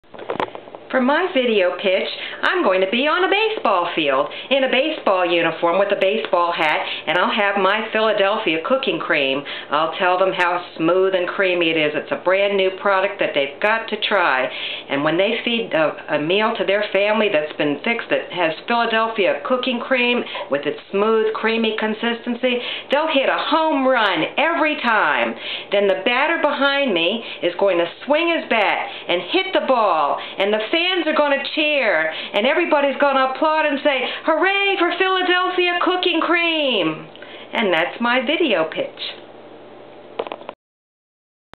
Video Pitch: Hit A Home Run With Philadelphia Cooking Creme!
My video pitch for a commercial for new Philadelphia Cooking Creme on a baseball field. Hit a home run every time dinner includes Philly Cooking Creme!